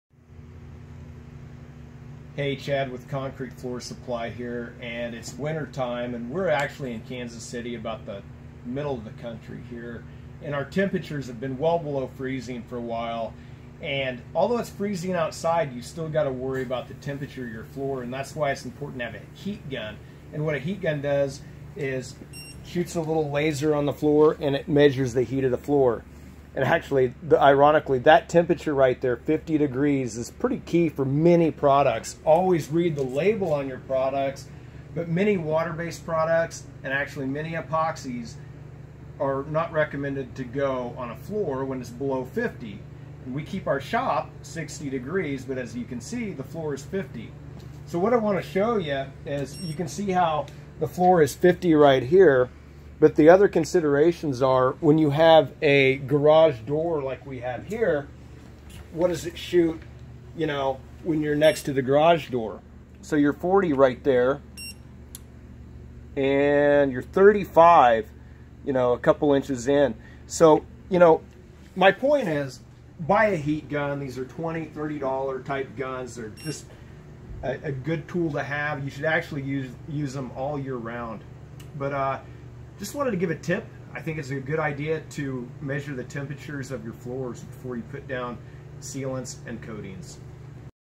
Here's Why Checking Your Concrete Floor Temperature is Important!
Checking concrete floor temperature before applying sealers and coatings to concrete.